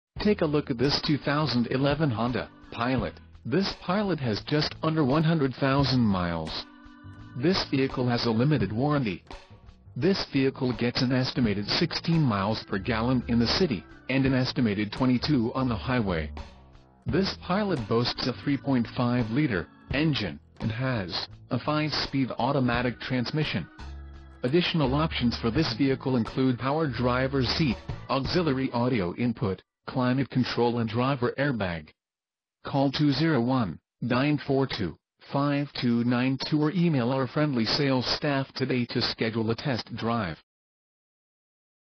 2011 Honda Pilot 4WD 4dr EX SUV - Jersey City, NJ
New Jersey State Auto Auction, Jersey City, NJ - Honda Pilot 4WD 4dr EX 3rd Row Seating